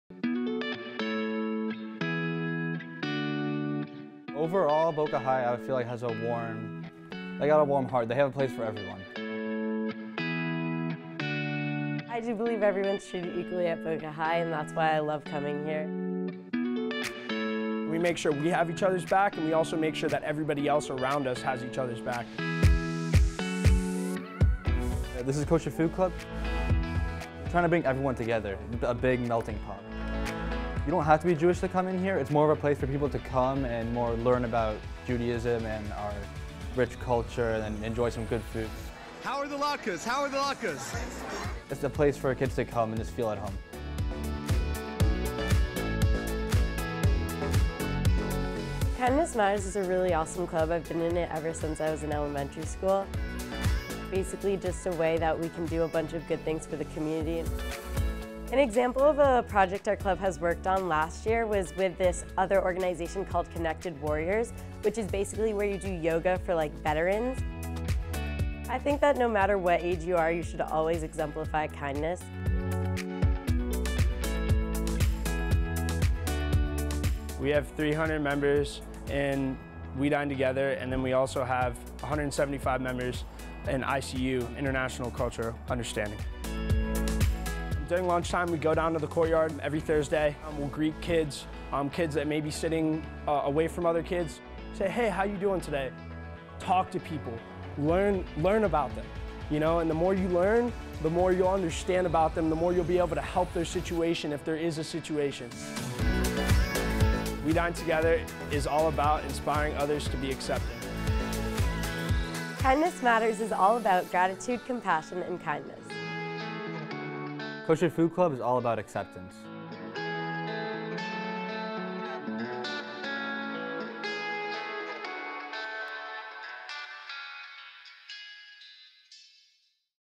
Boca Raton High School Clubs Promote Kindness, Inclusivity, Service
Boca Raton High School has at least 25 different clubs dedicated to serving others, promoting kindness and inclusivity, or discussing various cultures. Watch to see how the student leaders behind Kosher Food Club, Kindness Matters, We Dine Together, and International Cultural Understanding are leading the way of a positive, accepting school culture.